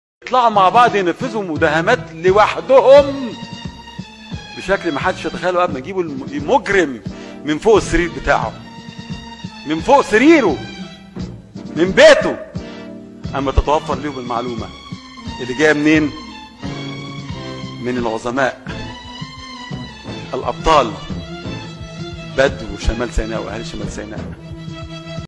شاهد ماذا قال وزير ||الدفاع المصري|| وشهادة عن ||بدو سيناء|| نور سيناء 🌴🇪🇬🌴
شهادة من وزير الدفاع المصري عن بطولات بدو سيناء في الدفاع عن أرضهم ووطنهم . اللهم احفظ مصر سيناء .

👈اللهم صل على سيدنااااا محمد ووووعلى اااااااااله وصحبه اجمعين وسلم تسليما كثيرا عدد خلقك ورضا نفسك وزنة عرشك ومداد كلماتك يارب العالمين 🌴🌴